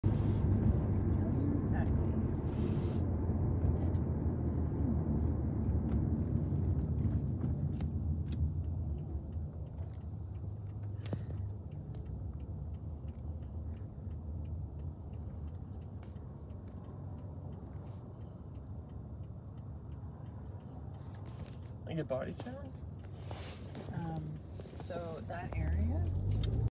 Multiple O.P.P. units on scene of an unknown emergency
What I am thinking that it was a body found because there is a homeless camp but if you think differently leave a comment of what you think it is